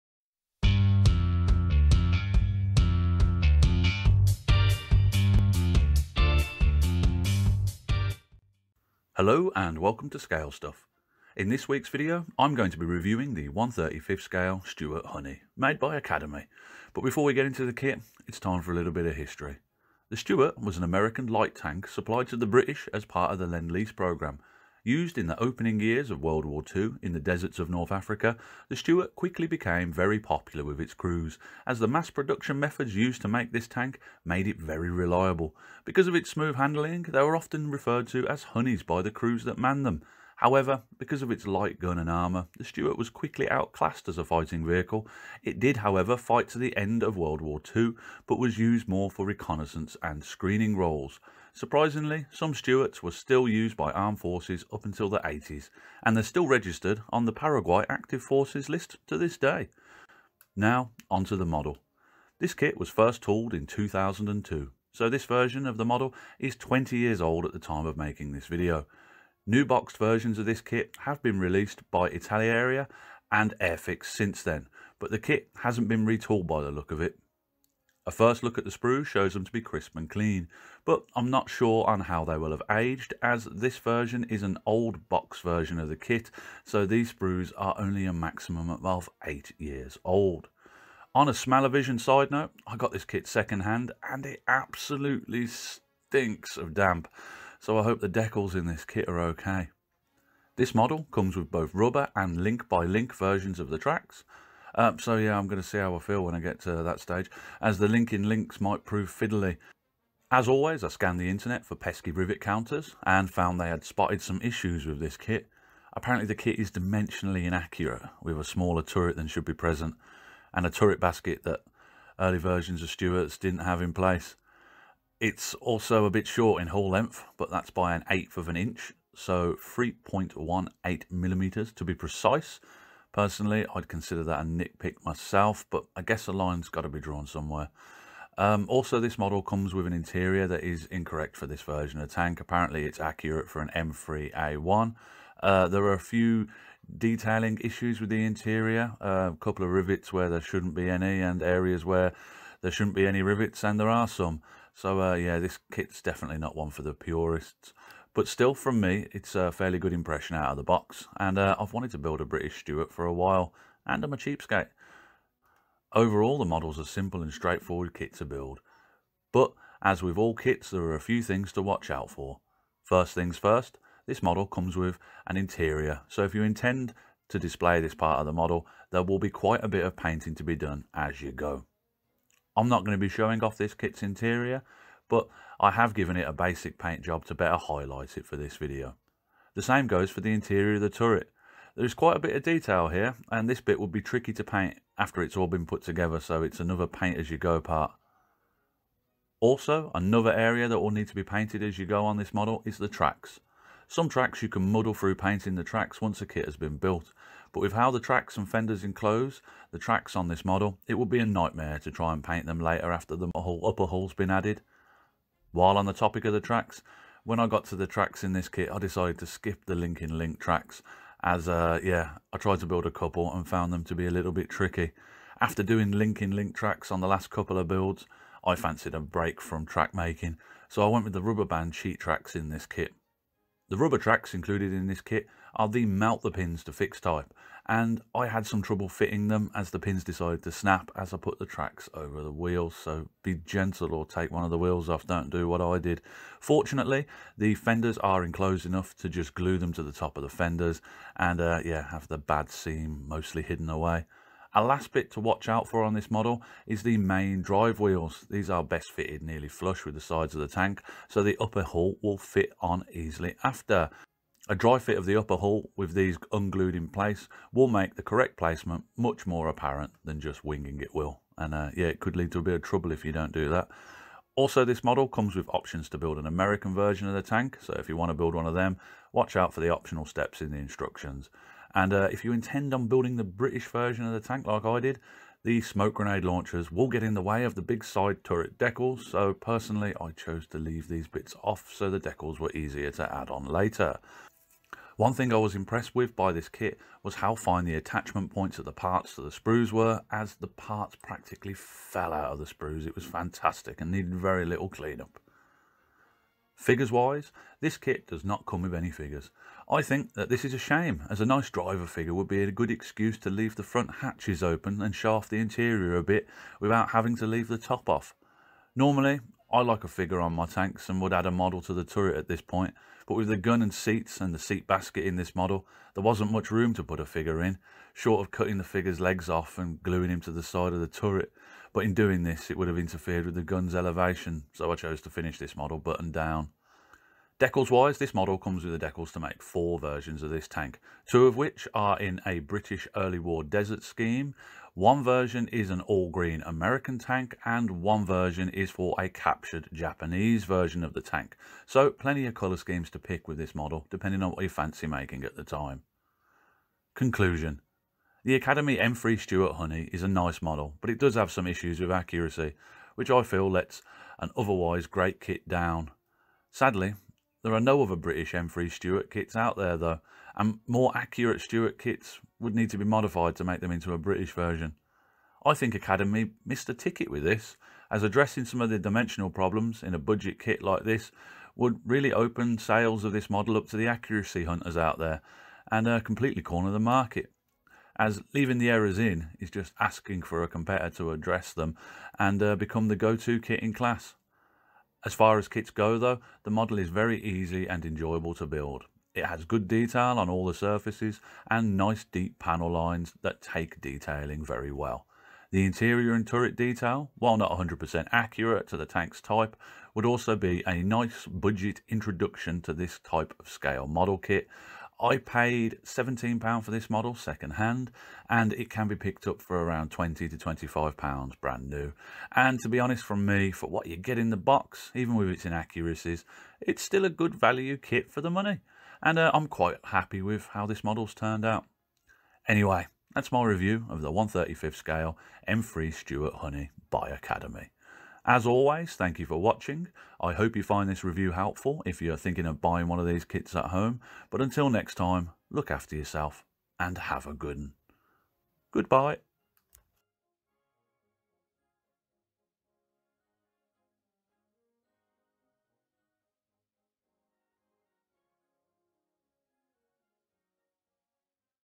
Academy 1/35 scale M3 Stuart Honey Build review
Hello and welcome to scale stuff in this weeks video I am going to be reviewing my build of the Academy 1/35 scale M3 Stuart Honey

Academy No. 1399

In this video I talk about how the kit goes together and point out a few things to watch when building this kit at home. I also look at the history of the kit and some inaccuracies that can be found in this scale model kit

As always I hope you find the video helpful if you are planning on building one of these model kits at home